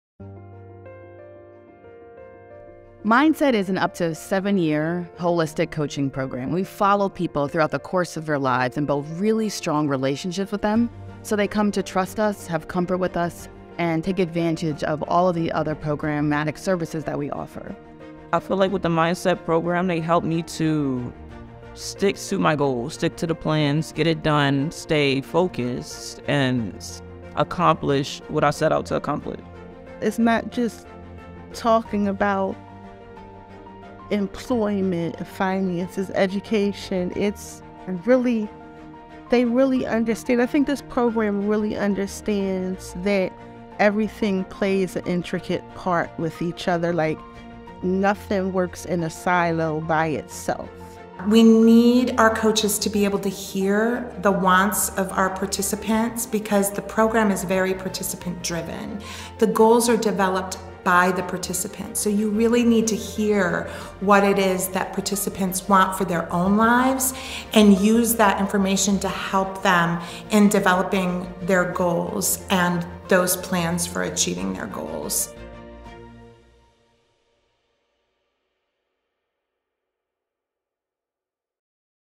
ECS Philly I Mindset Program I Career Development and Financial Stability
The ECS Philly Mindset program is a coaching and training program that aims to help residents of the Greater Philadelphia region achieve their goals, develop executive functioning skills, and attain self-sufficiency for themselves and their families.

This program combines one-on-one coaching with group workshops to provide participants with essential skills related to health and wellness, employment, education, and financial management. Mindset participants will be matched with a coach based on the coach’s experience and skillset, plus the participant’s availability and interests.

This program is unique as it offers a complimentary service for workforce training participants and alumni in transitional or entry-level employment to build family-sustaining careers with robust, affordable benefits. The program is free of charge and is provided by highly qualified specialists in the areas of education and employment.

Additionally, ECS will provide cash incentives for completing specific program activities and a 2 to 1 cash match on the money each individual saves within a designated bank account.

To be eligible for the program, applicants must be age 18 or older, reside in the Greater Philadelphia Area, be legally allowed to work in the USA, have at least three to five years of work experience, have graduated from high-school or have a GED, and be fluent in the ability to write and speak English.